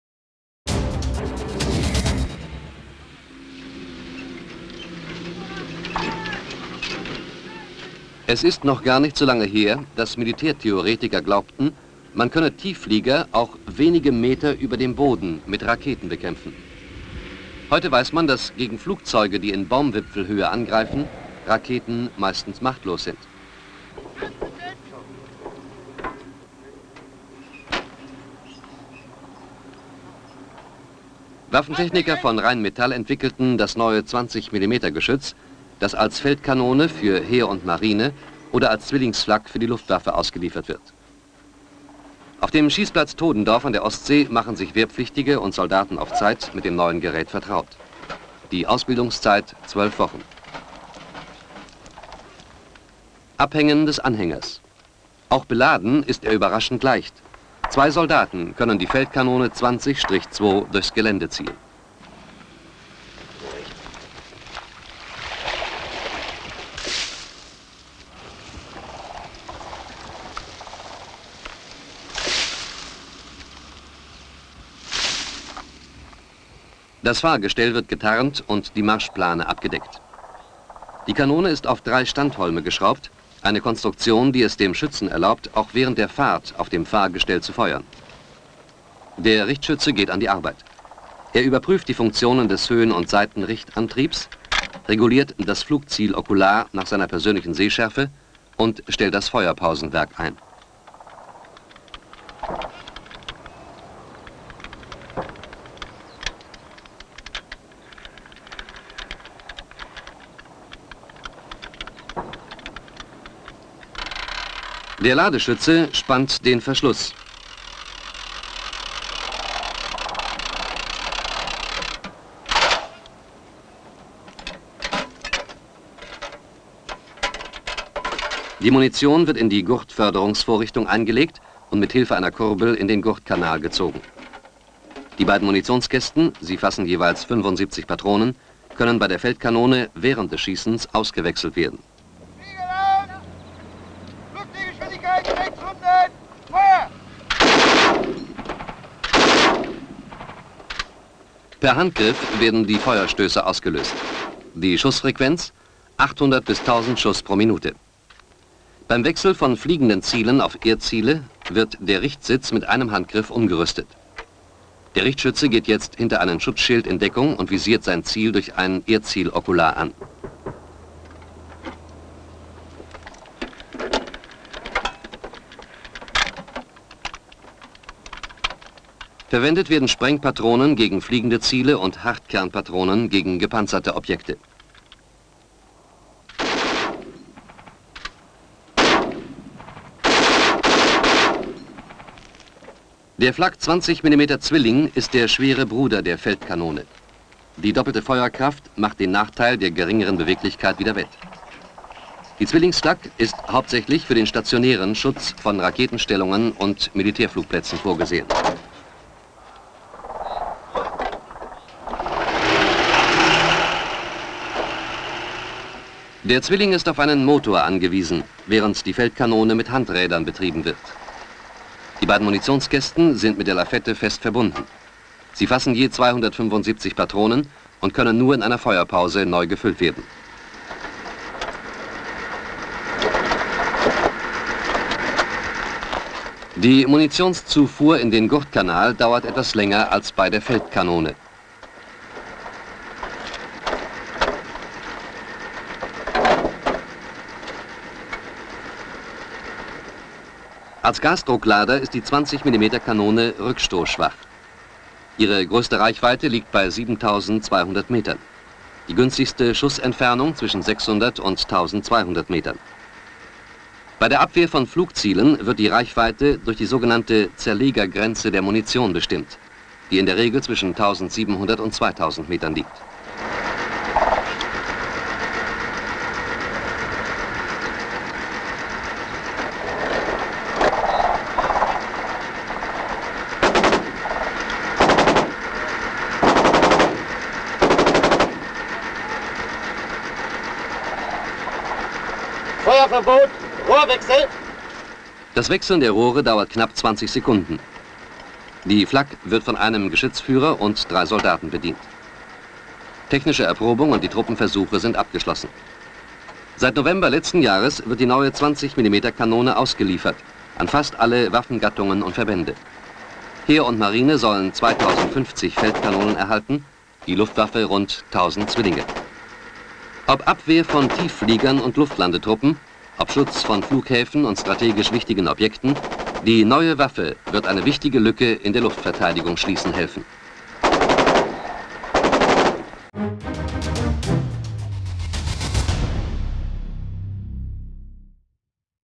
Classix: die Flak- und Feldkanone von Marine, Heer und Luftwaffe (1973) - Bundeswehr
Waffentechniker entwickelten eine neue 20 mm Schnellfeuerkanone. Das Heer und die Marine werden über 2000 solcher Kanonen als Feldkanone (Produktion von 1970 bis 1976) zur Bekämpfung von Tieffliegern und Erdzielen erhalten – und zwar als sogenannten „Einling“.
Die Luftwaffe erhält die gleiche Waffe als Zwillingssystem („Zwillling“) ab 1972 für den Schutz von Flugplätzen und strategisch wichtigen Objekten.

10/2018
18E23702